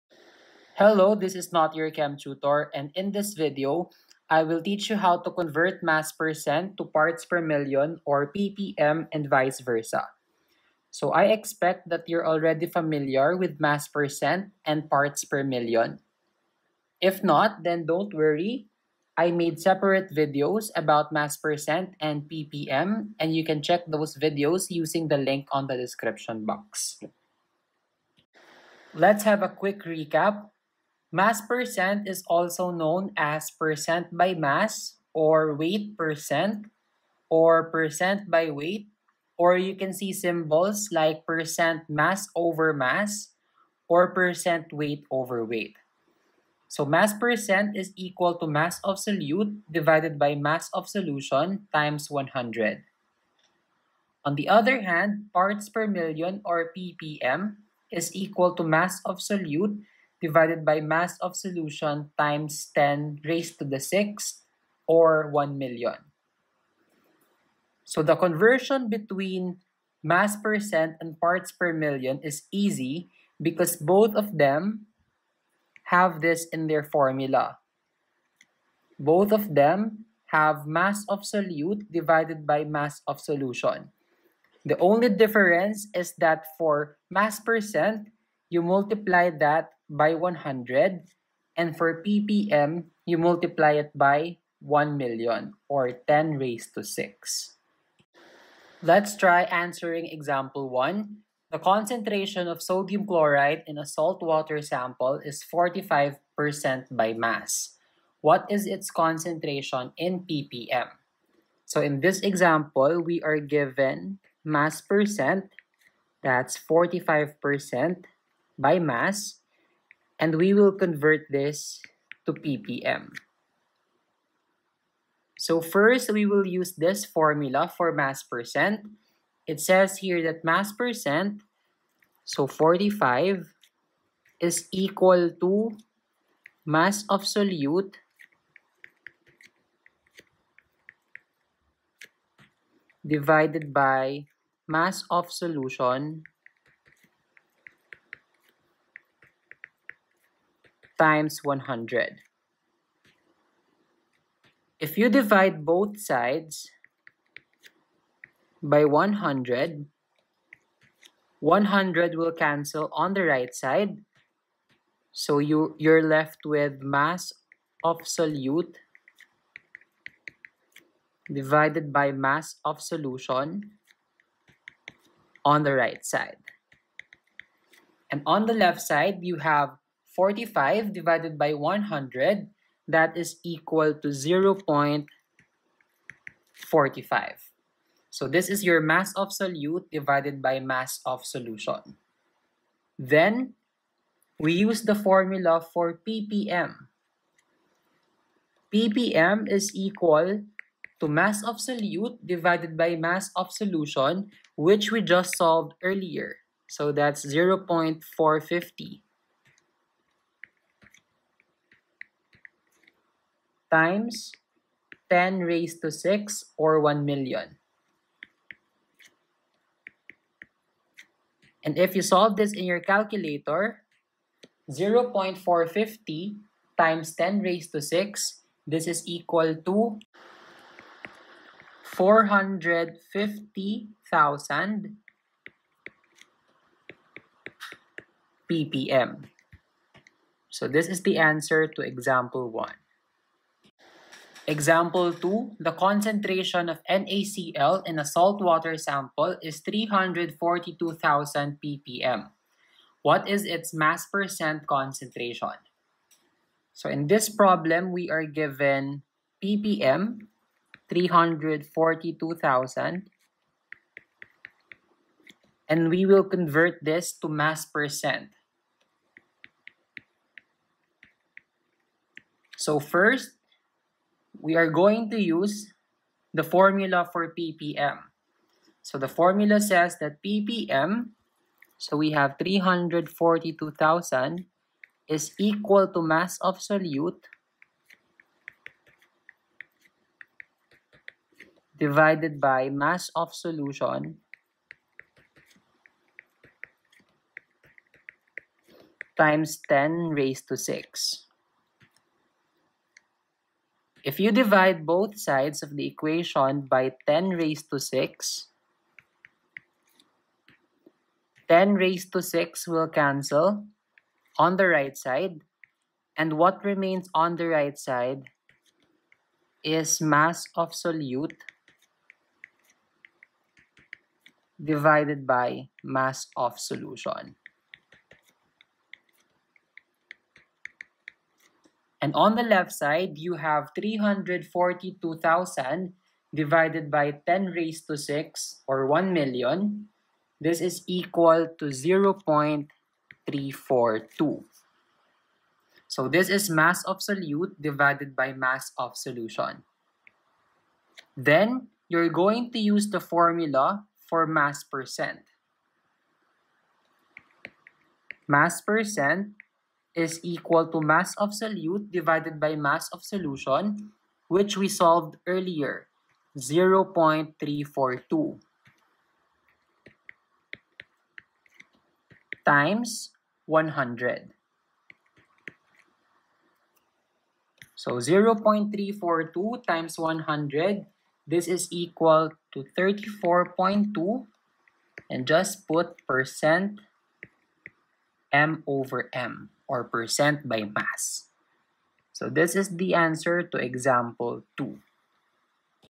HOW TO CONVERT MASS PERCENT TO PARTS PER MILLION (ppm) AND VICE VERSA?|WITH EXAMPLES AND EXPLANATION
NOTE that this video is about converting mass percent to ppm(m/m). Please take note that in my video about ppm, I talked about two formulas for ppm:
ppm(m/m) = mass solute/mass solution x 10^6
ppm(m/v)= grams solute/mL solution x 10^6